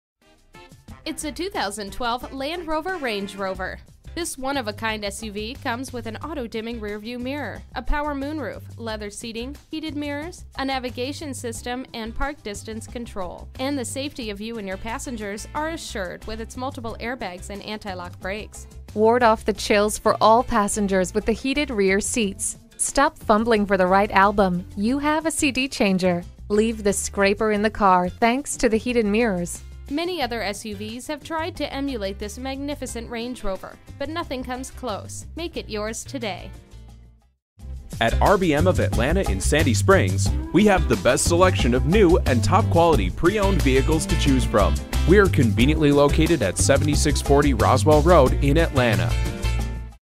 2012 Land Rover Range Rover Atlanta GA Marietta, GA M28781A - SOLD
Year: 2012
Make: Land Rover
Model: Range Rover
Trim: HSE LUX
Engine: 8 Cyl. 5.0L
Transmission: Automatic
Color: Indus Silver Metallic
Interior: Ivory
Mileage: 8672
Stock #: M28781A
ONE OWNER, CLEAN CARFAX! NAVIGATION, REAR SEAT ENTERTAINMENT, SUNROOF WITH SLIDING SHADE, harman/kardon SOUND SYSTEM, REAR CARGO SLIDING COVER, SPLIT FOLDING REAR SEATS, 3 ZONE CLIMATE CONTROL, REAR CARGO MAT, AND PUSH BUTTON START/STOP.

Address:
7640 Roswell Rd